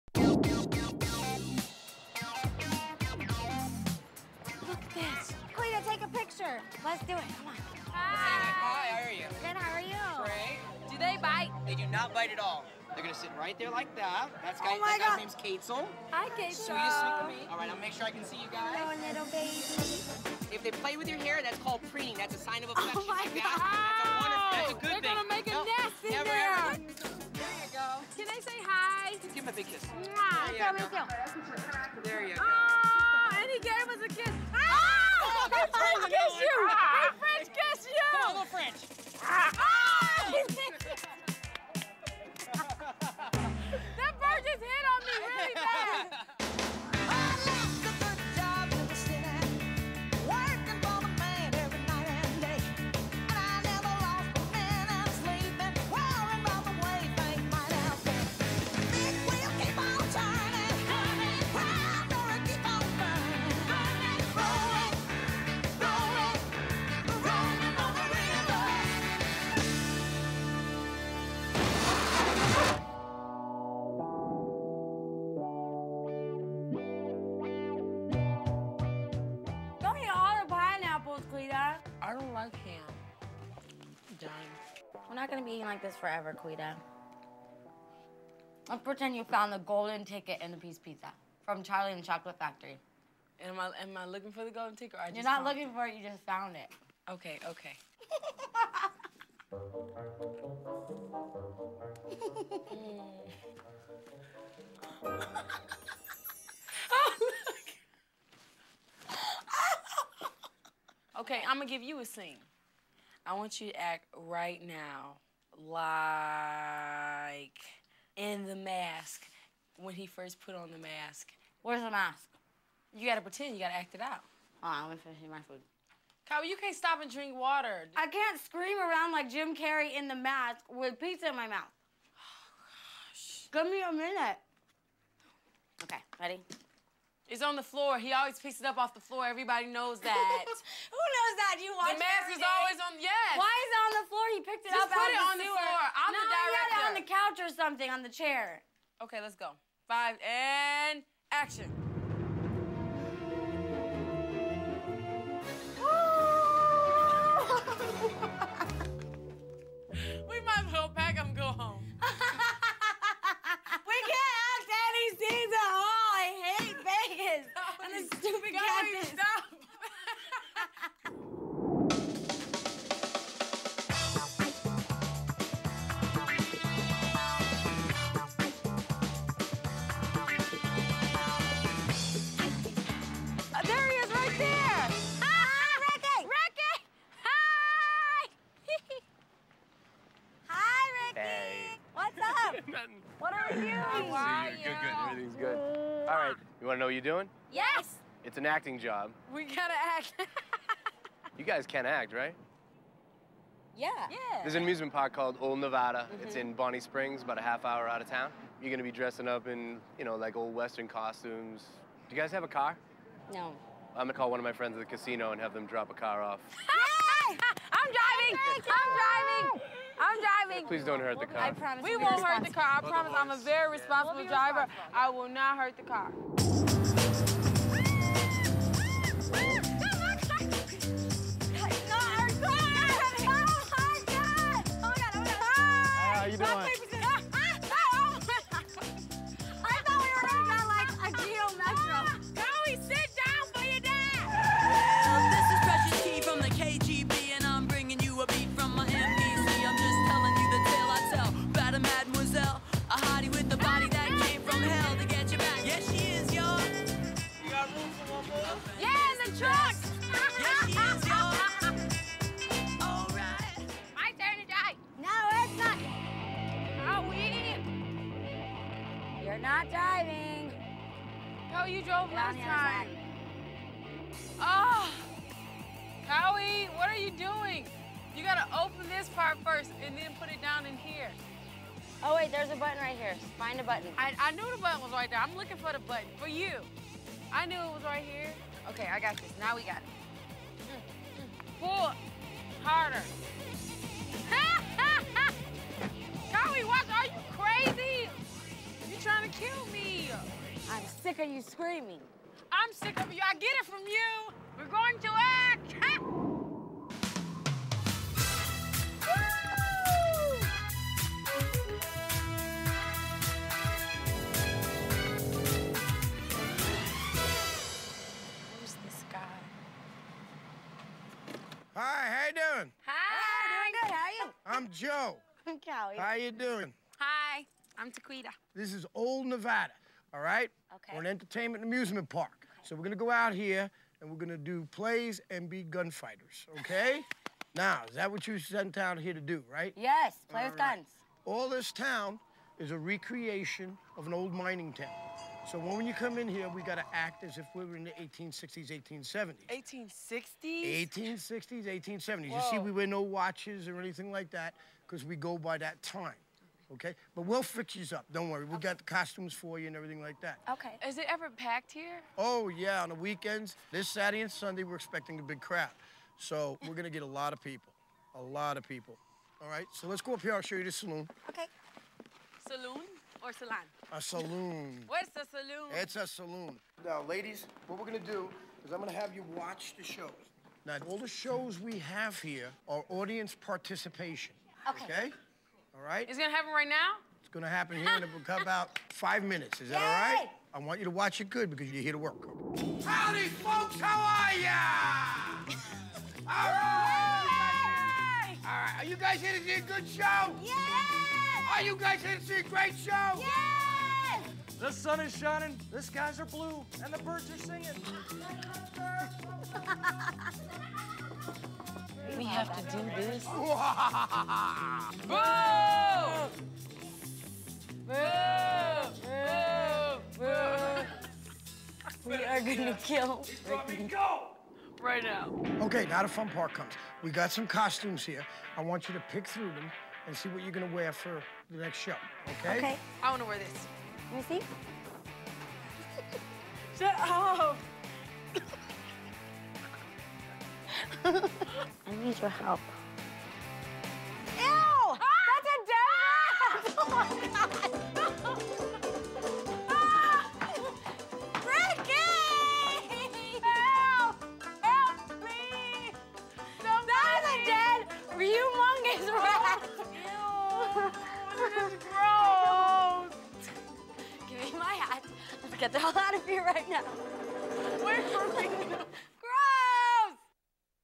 Taquita & Kaui Episode 3 Part 1
The girls work as actresses in an old Western theme park.  Taquita wrecks their new corvette.